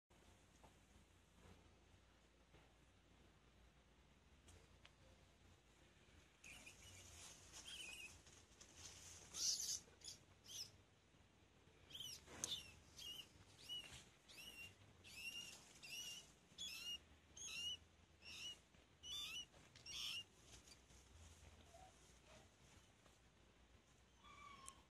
Orange Cat Catching a Rat
Kitten catches a rat